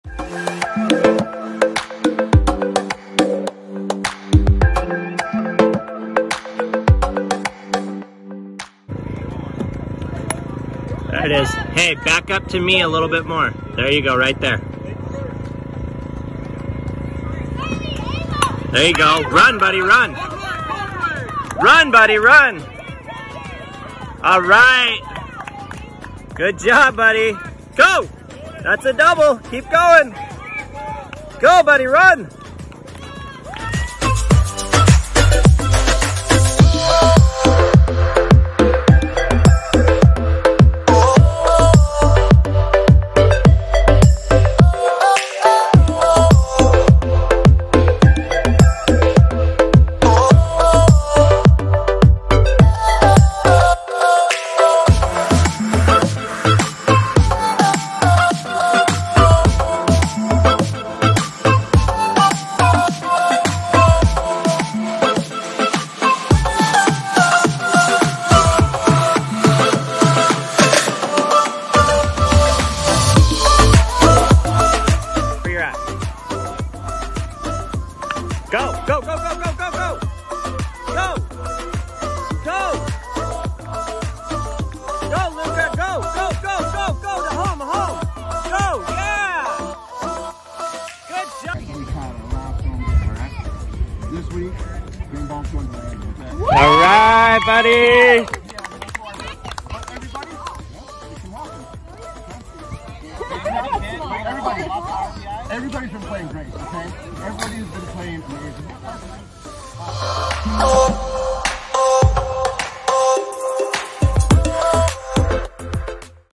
My Epic Baseball Game
I am loving baseball. One of my last games I did so good that I got the game ball!!